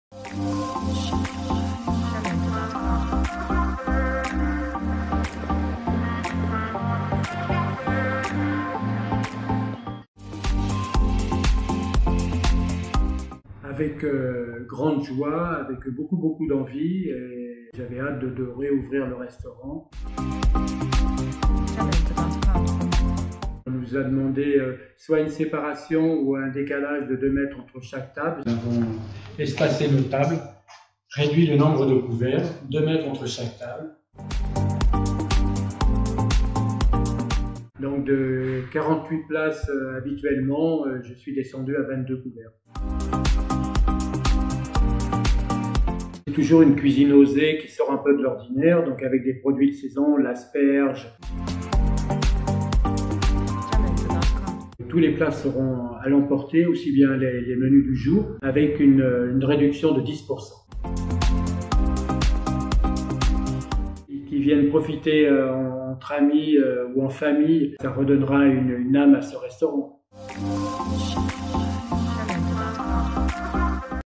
[JeSoutiensMonResto] Connaissez-vous le Café des Banques?
Deuxième volet de l'opération JeSoutiensMonResto de la CCIG avec la présentation du Café des Banques.